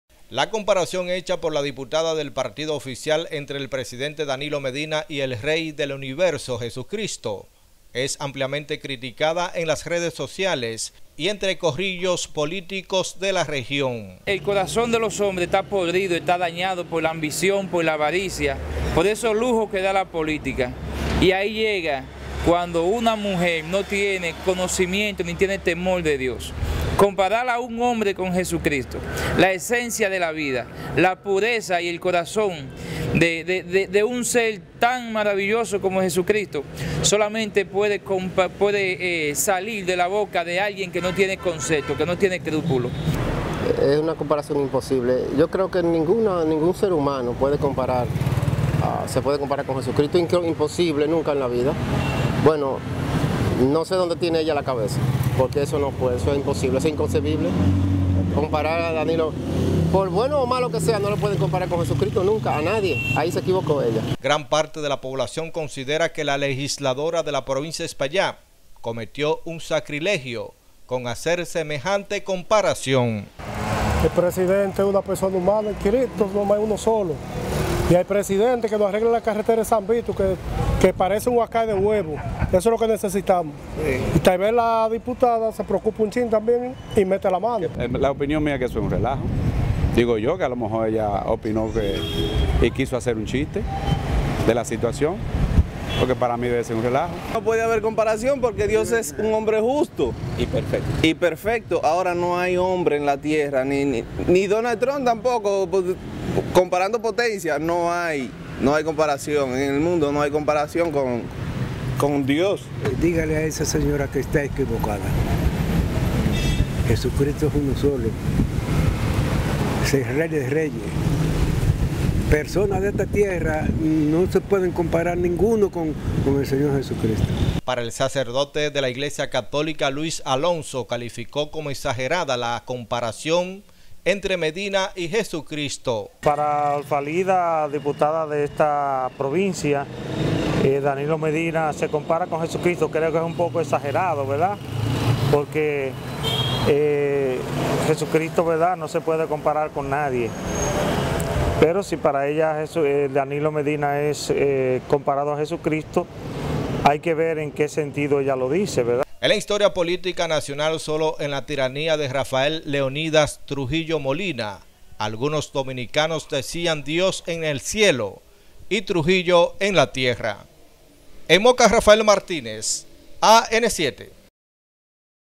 Critican diputada por comparar a Cristo con Medina
Moca /05/03/2018
Rafael Martínez/Duración  / 2: 20
Historia/ titulo: Critican diputada

Un arcoíris de opiniones generó en poblaciones del Cibao la comparación entre el Presidente de la República Danilo Medina con Jesucristo hecha por la diputada Olfalida Almonte del Partido de la Liberación Dominicana.